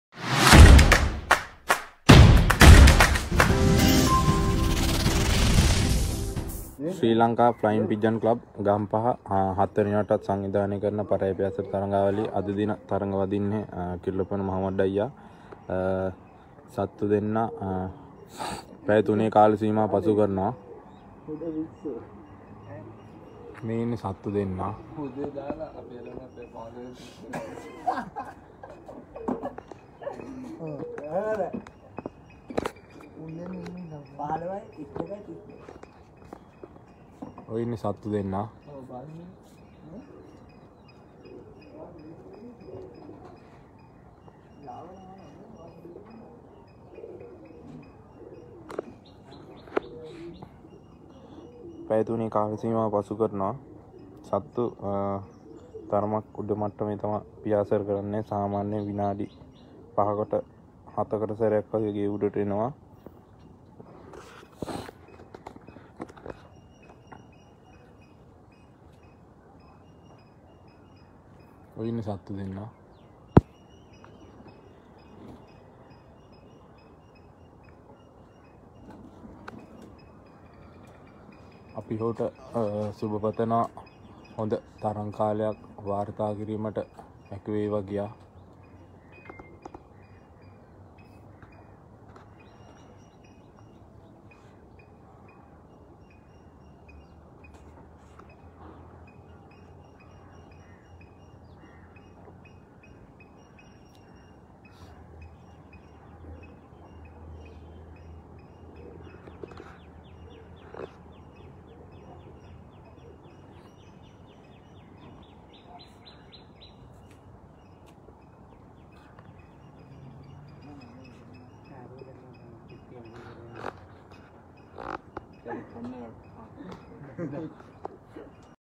today pigeon competition - pigeon loft in Kirulapana. Mohammed සතුන් දෙදෙනා පැය 03 කාල සීමාව පසු කළහ
We congratulate Mohammed Rizeel Kirulapana who released the animals at 05.00 a.m. today in the 7th pigeon flying tournament organized by the SRI LANKA FLYING PIGEON CLUB Gampaha Association and we congratulate him for recording a good competition time...

SRI LANKA FLYING PIGEON CLUB Gampaha සංගමය විසින් සංවිධානය කරන ලද 7 වන පරෙවි පියාසර තරඟාවලියේ දී අද පෙරවරු 05.00 ට සතුන් මුදා හරින මොහොමඩ් රිසීල් කිරුළපන මහතාට අපගේ සුභාශිංසන පිරිනමන අතර හොඳ තරඟ කාලයක් වාර්තා කිරීම ගැන අපි ඔහුට සුබ පතන්නෙමු...

Congratulations  💪💪💪